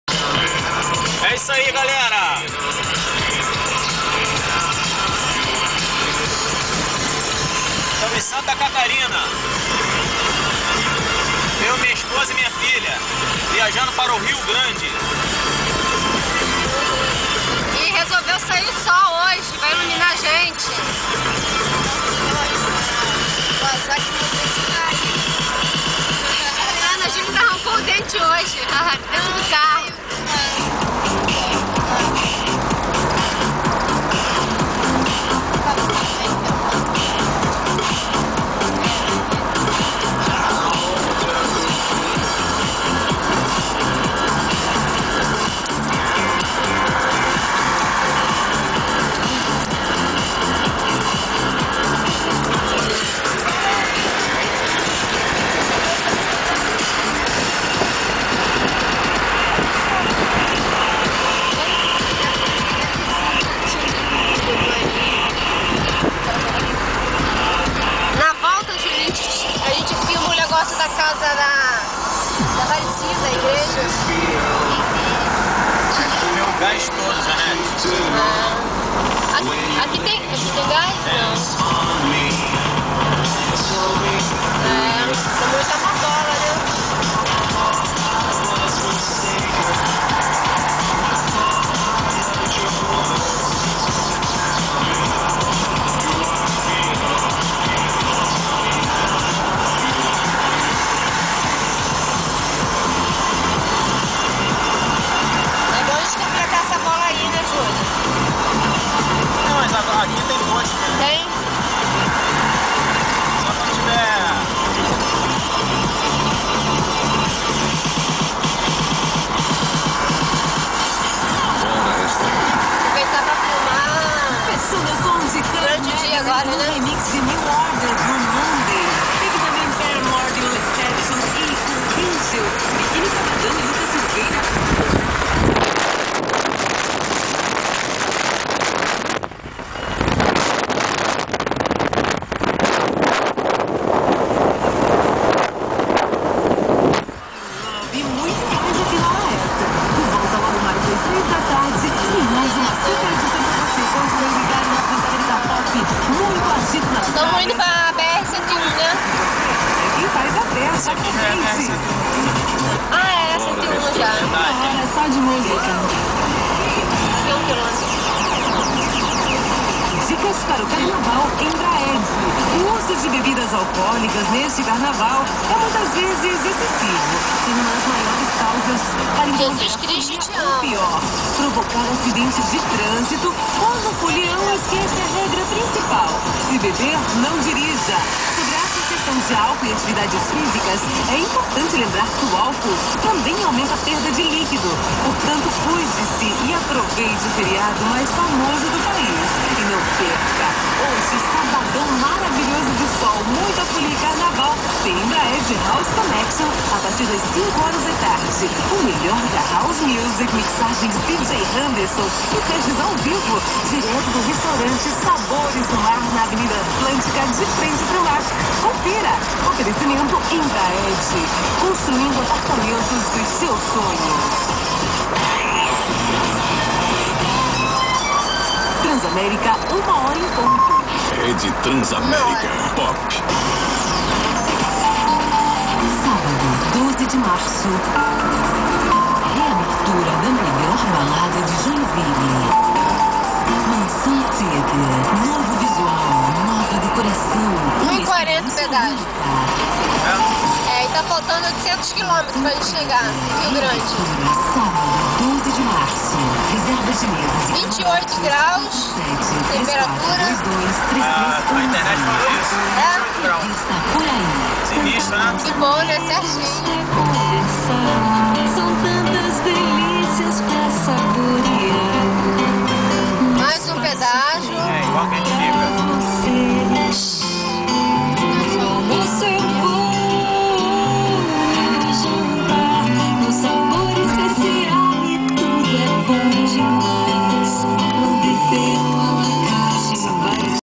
Rio de Janeiro Uruguay 23
Viagem de Carro , do Rio de Janeiro para o Pais Uruguay , Realizada na data do carnaval de 2011 , Mais de 6000 mil Km Percorridos , três dias de viagem , com a primeira parada em Joinville sc no Hotel 10 , segundo dia na cidade do Rio grande rs , terceiro dia em Chuy e Punta del Diablo Uruguay, LInda Viagem , Vale a pena assistir todos os Videos.